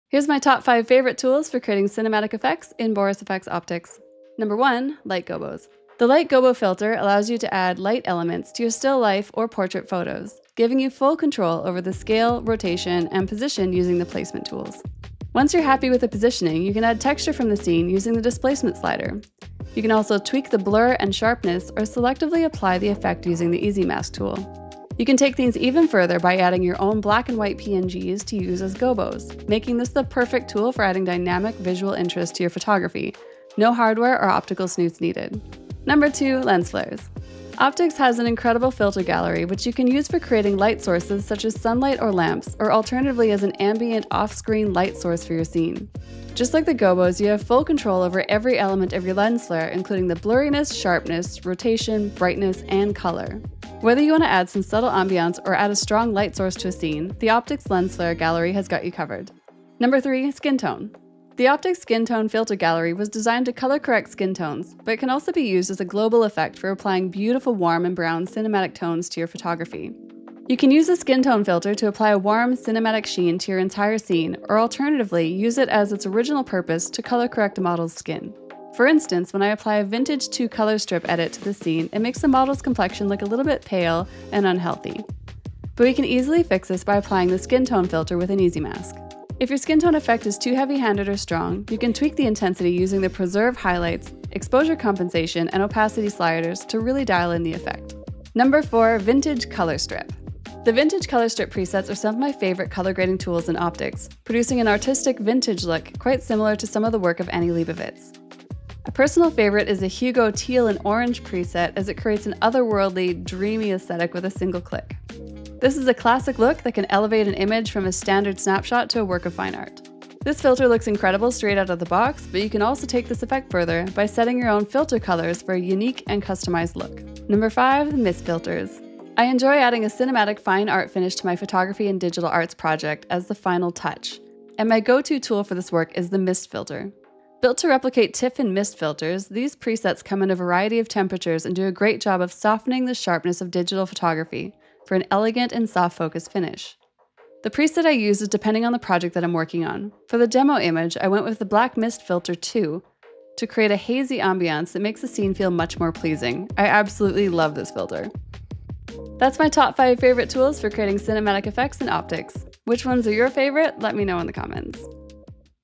Top 5 Tips for Cinematic Portrait Edits in Optics
Love cinematic photography? Here are our 5 favourite tools for editing cinematic portraits in BorisFX Optics!